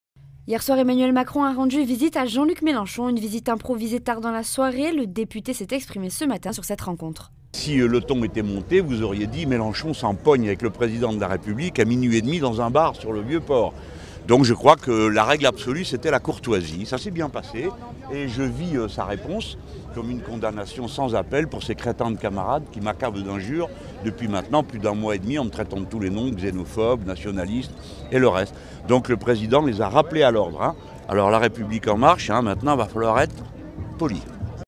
Marseille : "La règle absolue était la courtoisie", Mélenchon réagit à sa rencontre avec Macron
Ce vendredi soir, Emmanuel Macron a rencontréJean-Luc Mélenchon, député LFI des Bouches-du-Rhône, dans un établissement de Marseille. Une visite improvisée tard dans la soirée, après que le président de la République se soit entretenu avec la Chancelière allemande Angela Merkel. Ce samedi midi, en marge de la marche citoyenne pour la planète sur le Vieux-Port, Jean-Luc Mélenchon est revenu sur cette rencontre.